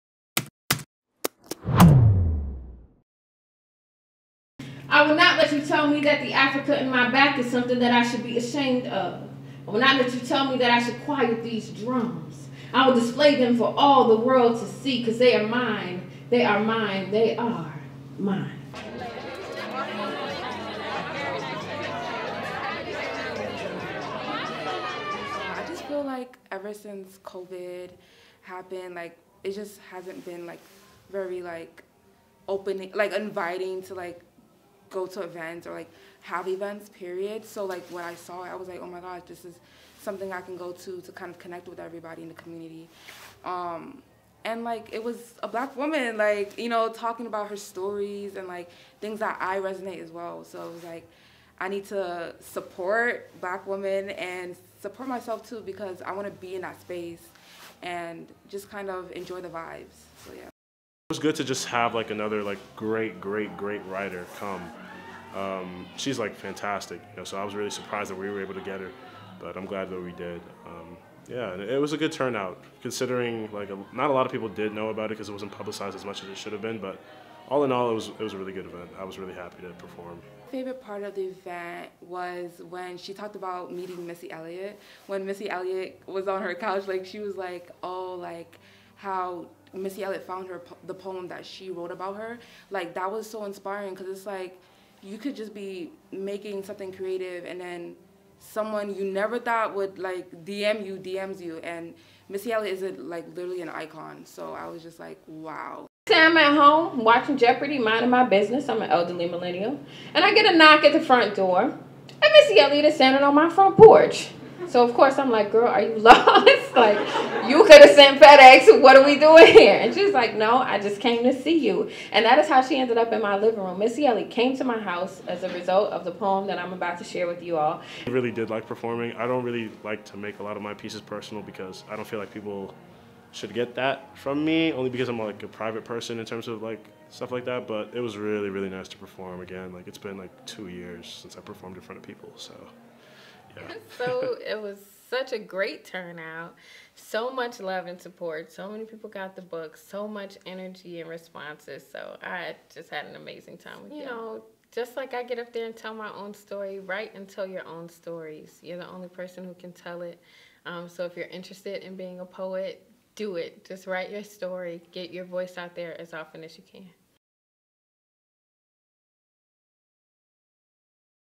Students Connect Through Poetry
On Feb. 8 in IC Square, spoken-word artist Ashlee Haze gave a poetry performance following an opening from two student-poets.